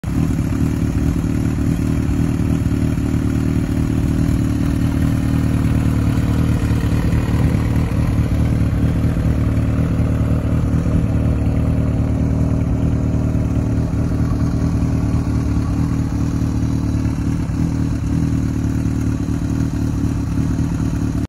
1978 Kawasaki KZ650 Restored!
We recently picked up a fully restored 1978 Kawasaki KZ650, and are instantly in love!
Smokes a little, but hey, she’s old, she can smoke if she wants to!
Story and more videos coming soon.